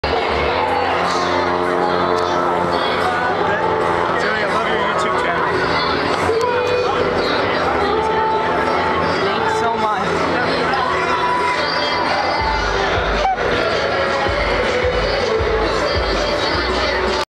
Sione Pouha Signing Autographs
Sione Pouha of the New York Jets signs autographs for fans before the game against the Chargers at MetLife Stadium on October 23, 2011.